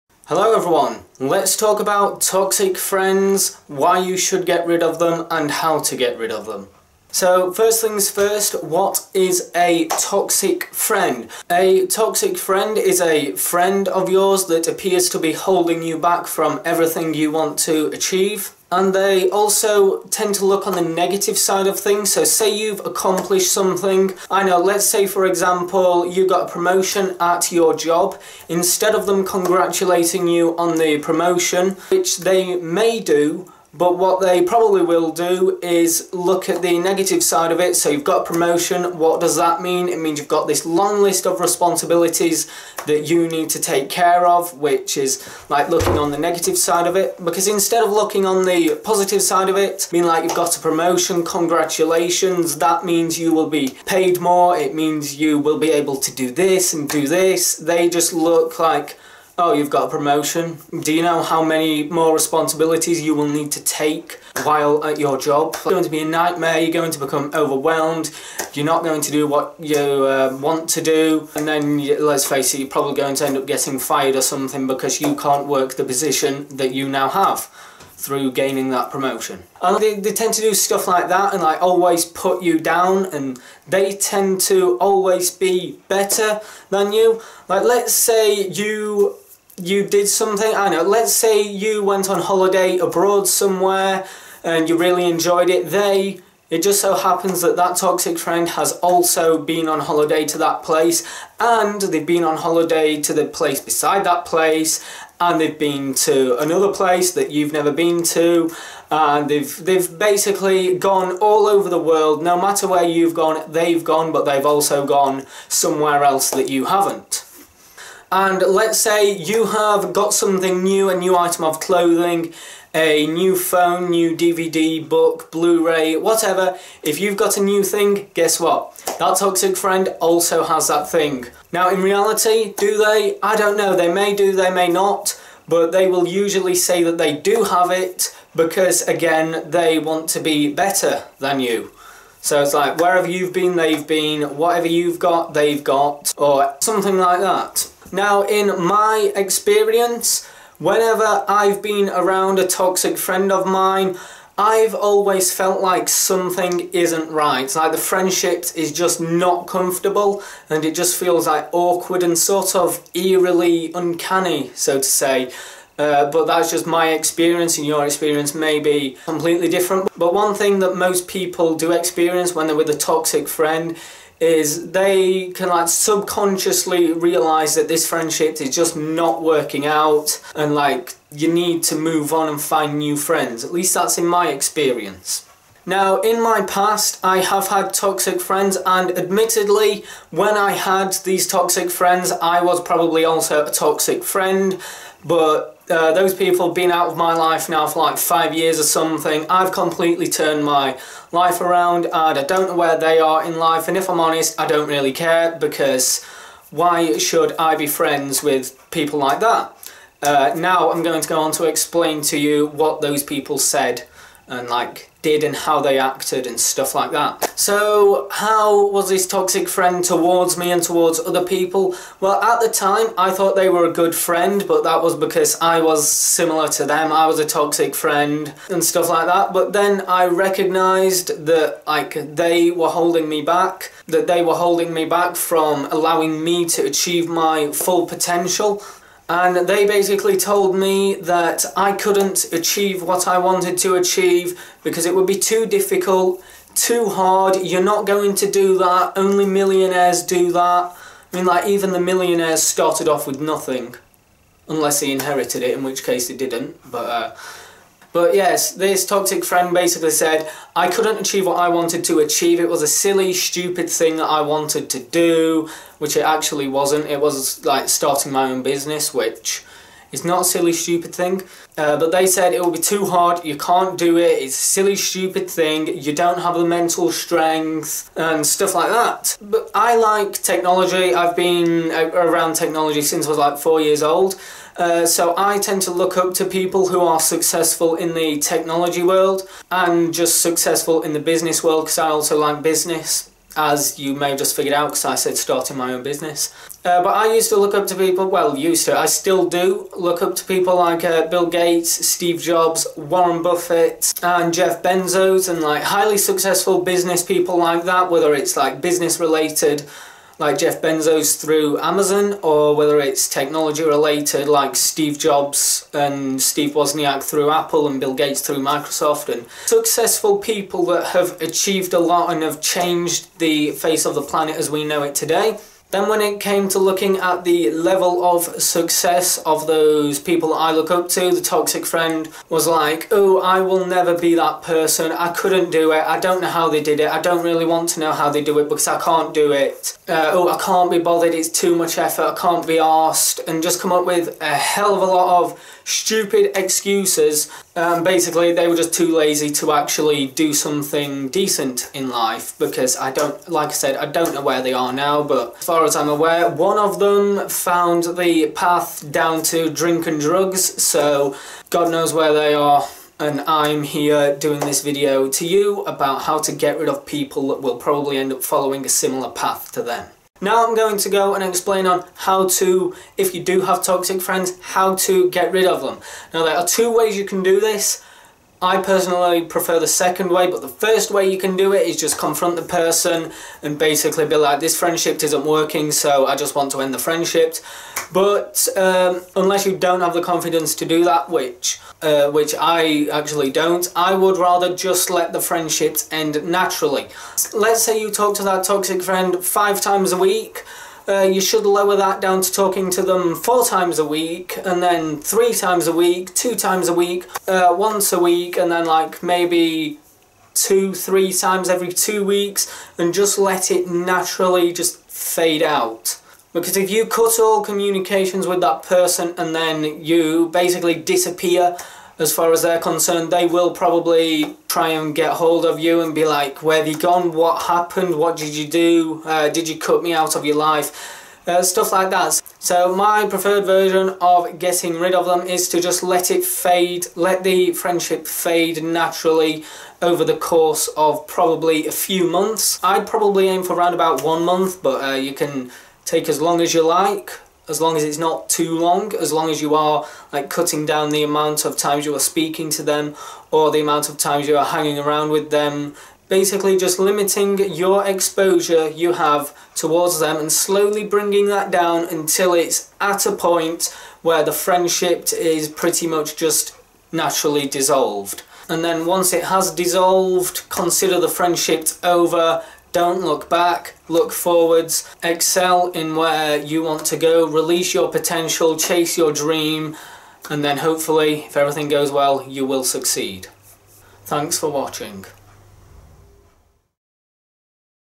Talk: Toxic Friends - How To Identify & Remove Them From Your Life!
So this is an idea I've had for ages - talking about toxic friends, how you can identify them, a few of my experiences I've had with them in the past, and finally, how to remove them from your life in order to better your life for the future.